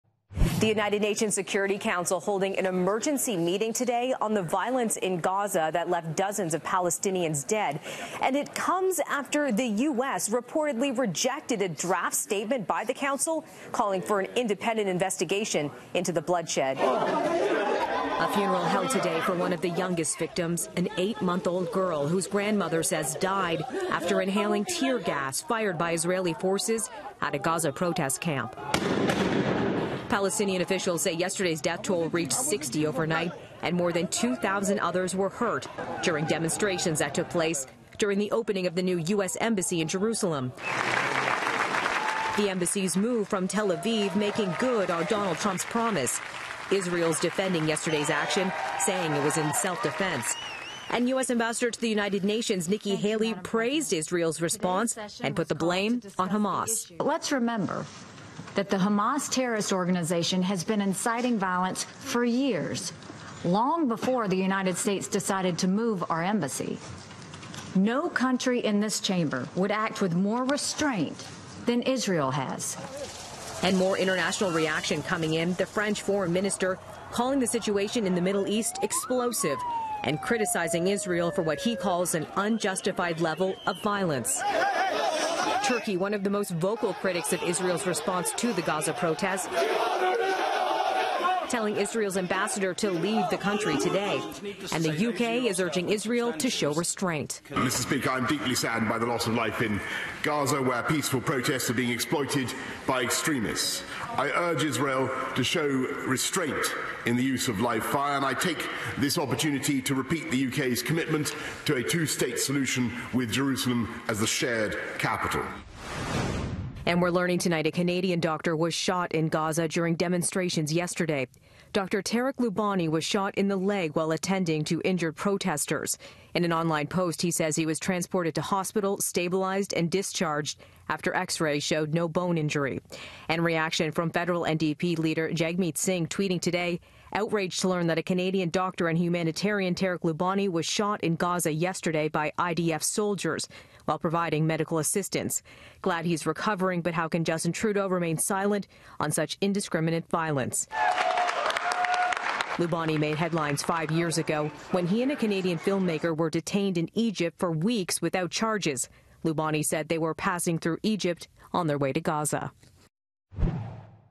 U.N. Security Council holds emergency meeting following deadly Gaza protests
The United Nations Security Council held an emergency meeting Tuesday in the wake of the deadly protests in Gaza. Meanwhile, Canadian doctor Tarek Loubani is recovering after being shot while helping protesters during the violence on Monday.